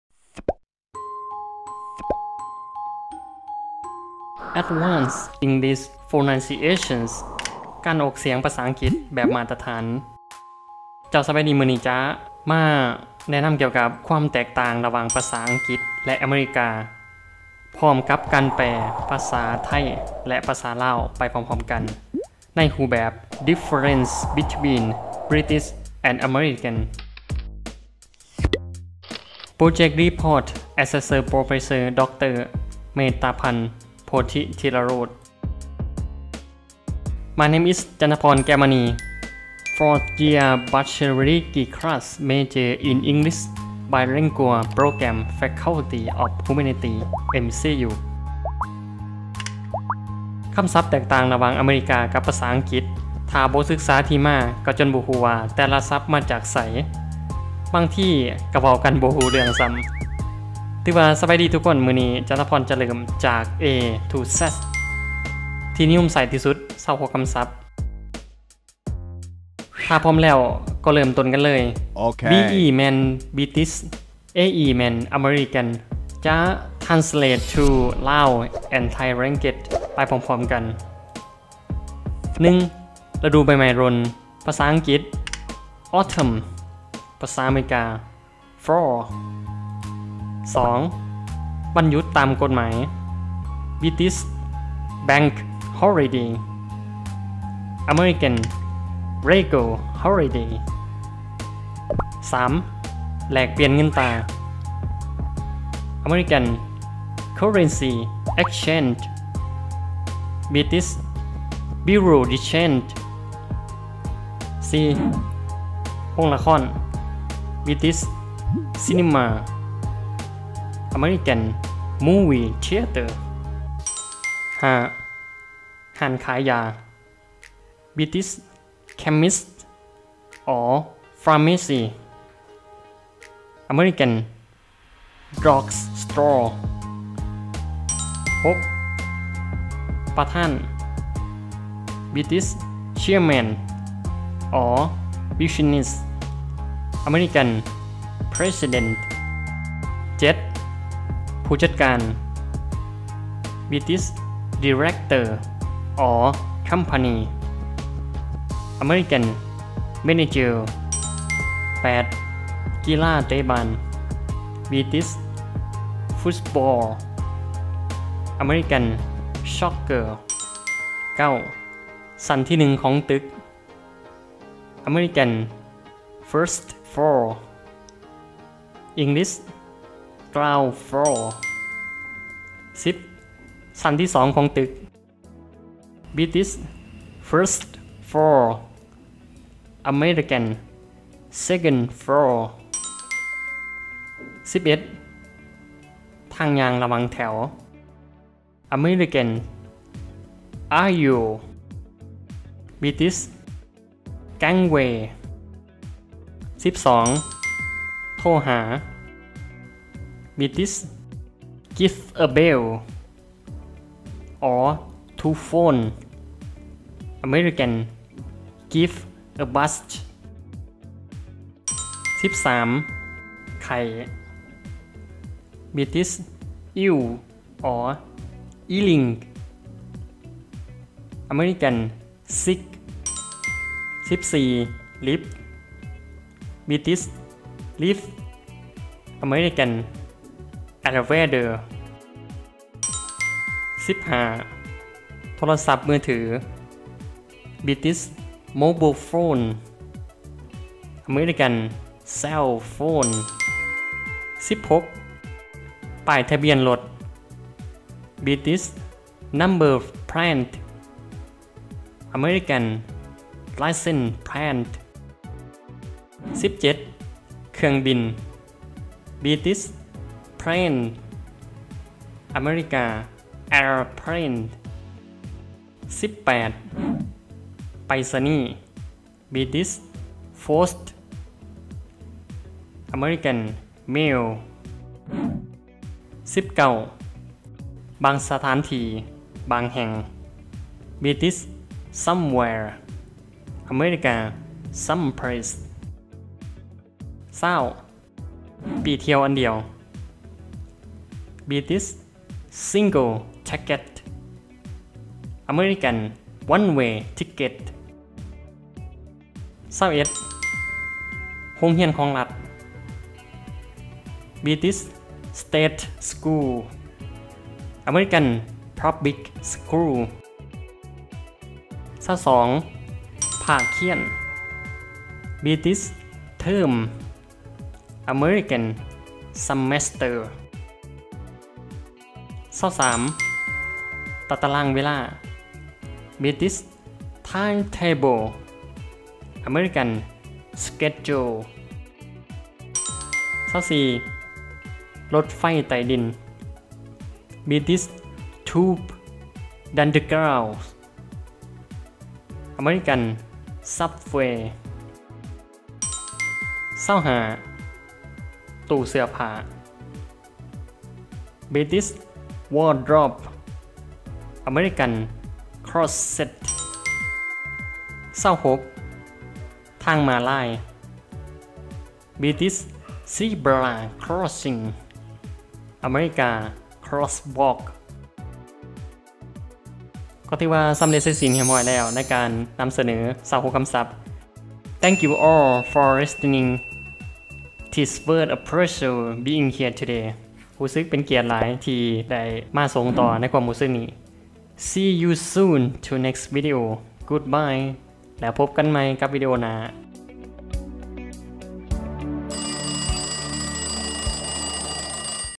ປຽບທຽບ เปรียบเทียบ British vs American LAO to Thai Language
autumn fall ฤดูใบไม้ร่วง ລະດູໃບໄມ້ລົ່ນ bank holiday legal holiday วันหยุดตามกฎหมาย ວັນຫຍຸດຕາມກົດໝາຍ bureau de change currency exchange แลกเปลี่ยนเงินตรา ແລກປ່ຽນເງິນຕາ Cinema (British) vs Movie Theater (American) โรงภาพยนตร์ ໂຮງລະຄອນ chairman (business) president ประธาน ປະທານ director (company) (British) manager (American) ผู้จัดการ ຜູ້ຈັດການ Football (British) vs Soccer (American) กีฬาฟุตบอล ກີລາເຕະບານ Ground floor (British) vs First floor (American) ชั้นที่หนึ่งในตึก ຊັ້ນທີ່ 1 ຂອງຕຶກ First floor (British) vs Second floor (American) ชั้นที่สองของตึก ຊັ້ນທີ່ 2 ຂອງຕຶກ gangway (British) aisle (American) ทางเดิน ທາງຍ່າງ give a bell (to phone) give a buzz โทรหา ໂທຫາ ill (British) vs Sick (American) ป่วย ໄຂ້ Lift (British) vs Elevator (American) ลิฟต์ ລິບ Mobile phone (British) vs Cell phone (American) โทรศัพท์มือถือ ໂທລະສັບມືຖື Number plate (British) vs License plate (American) ป้ายทะเบียนรถ ປ້າຍທະບຽນລົດ Plane (British) vs Airplane (American) เครื่องบิน ເຄື່ອງບິນ Post (British) vs Mail (American) ไปรษณีย์ ໄປສະນີ Somewhere (British) vs Someplace (American) บางที่, บางแห่ง ບາງສະຖານທີ່, ບາງແຫ່ງ single ticket one-way ticket ตั๋วเที่ยวเดียว ປີ້ທ່ຽວອັນດຽວ state school (British) public school (American) โรงเรียนของรัฐ ໂຮງຮຽນລັດ Term (British) vs Semester (American) ภาคการศึกษา ພາກຮຽນ Time-table (British) vs Schedule (American) ตารางเวลา ຕາຕະລາງເວລາ Tube / Underground (British) vs Subway (American) รถไฟใต้ดิน ລົດໄຟໃຕ້ດິນ Wardrobe (British) vs Closet (American) ตู้เสื้อผ้า ເສື້ອຜ້າ Zebra crossing (British) vs Crosswalk (American) ทางม้าลาย ທາງມ້າລາຍ